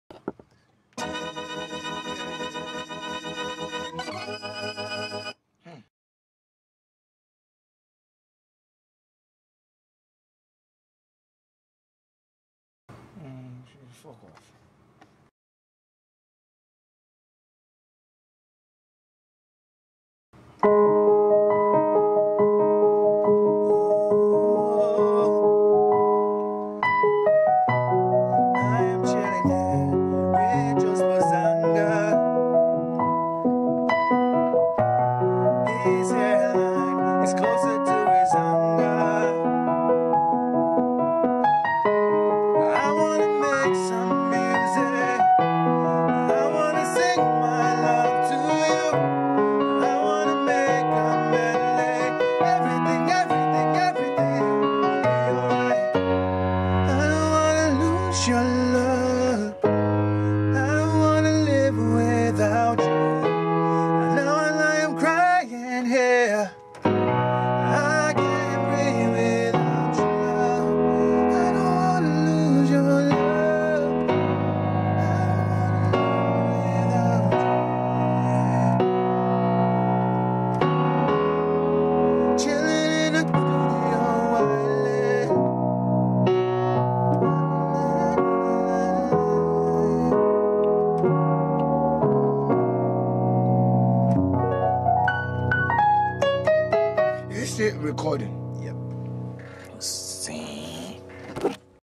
Sessions Vol 1.
Joespo Stringer aka Joseph Eyigiyeh on the keyboard
Me doing a little freestyle ting in the background

I've been making music with ths breddah for almost 10 years
he is one producer and fellow artist i just got this amazing relationship with
we hear music with the same ear
we have made many awesome tracks over the years - many of which have been lost or discarded because I wasnt in the place I am now
But once again we hit the studio a month or two ago and I've come back with a piece of art I can share with you
I will debut this track next year, but for now enjoy one of our freestyle song-writing sessions